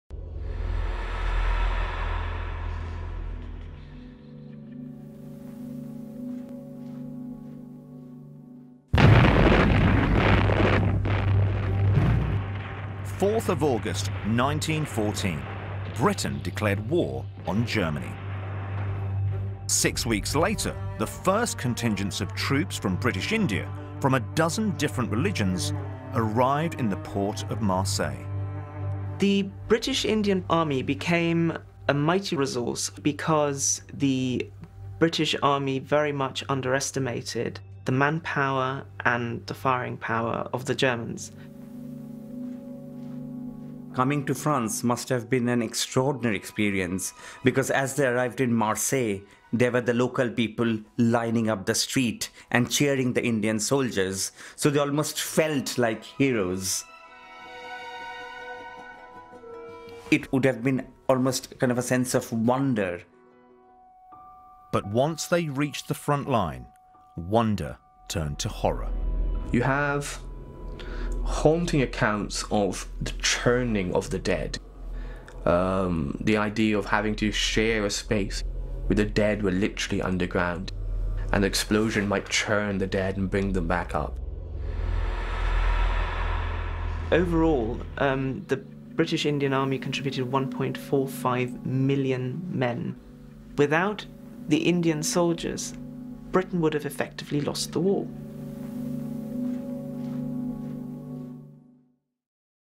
World War I: The role of Indian Army in Britain's victory
నవంబర్ 11, మొదటి ప్రపంచ యుద్ధం ముగింపు రోజు. ఆ యుద్ధంలో భారత సైన్యం పాత్ర ఎలా ఉండేది? బ్రిటన్ గెలుపునకు అది ఎలా దోహదపడింది? ఆనాటి సైనికుల త్యాగాలను స్మరించుకుంటూ బీబీసీ కరెంట్ అఫైర్స్ అందిస్తున్న ఈ వీడియో చూడండి.